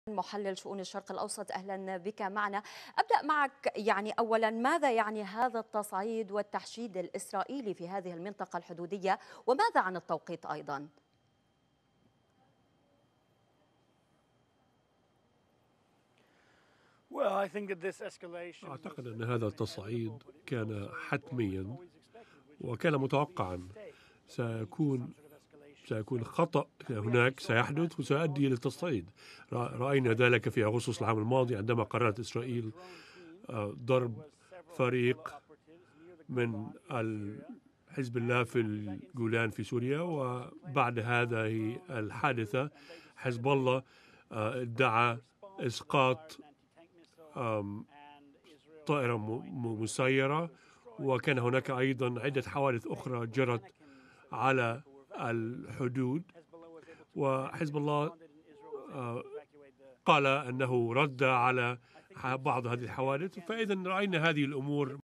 Seth Frantzman on Al-Hurra July 26 2020
Discussing Hezbollah tensions